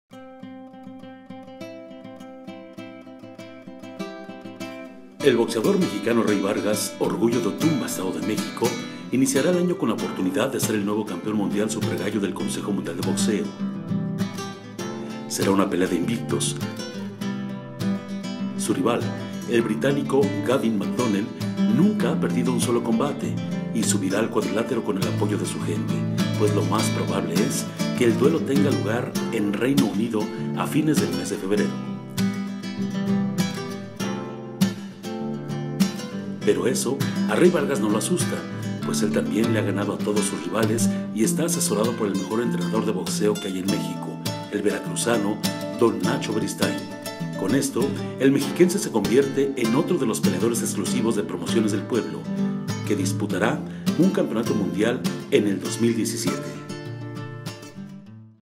REY VARGAS VS GAVIN MCDONNELL
VARGAS VS McDONELL POR EL CAMPEONATO MUNDIAL WBC
El invicto mexiquense Rey Vargas estará disputando el título mundial supergallo del Consejo Mundial de Boxeo frente al británico Gavin McDonell en el mes de febrero luego de que el japonés Hozumi Hasegawa, vigente campeón anunciara su retiro de los encordados.
El Consejo Mundial de Boxeo de acuerdo a su reglamento dio luz verde para que el mexicano, quien es el clasificado mundial número uno y retador mandatorio a la corona del asiático se enfrente al más alto clasificado disponible, caso de McDonell quien es el dos del mundo según las clasificaciones del prestigiado organismo verde y oro.
McDonell, de Yorkshire, Inglaterra; tiene 30 años y acumula una marca de 16 triunfos sin derrota con cuatro nocauts a favor y dos empates conquistando a lo largo de su carrera los títulos de la European Boxing Union y el cinto Plata WBc en la escala de las 122 libras, lo que le ha valido al hermano del campeón mundial gallo Jamie McDonell para colocarse en la cima de las clasificaciones.
Vargas a sus 26 años presume una marca invicta de 28-0 con un 80% de efectividad noqueadora; dominando desde que era un novato la división de los 55.330 kilogramos, acumulando en su palmarés las fajas mundiales Juveniles WBC e IBF y el título Internacional WBC que ha colocado al orgullo de Otumba, Estado de México; como el contendiente principal a la corona desde inicios de año pasado.
Vargas, avanzado discípulo de Don Nacho Beristáin había esperado este momento y ahora se encuentra ante la oportunidad de conquistar su sueño, con lo que se convierte en otro de los peleadores exclusivos de Promociones del Pueblo que se coloca en la antesala del título mundial, en un combate que podría llevarse a cabo el 18 o 25 de febrero en el Reino Unido.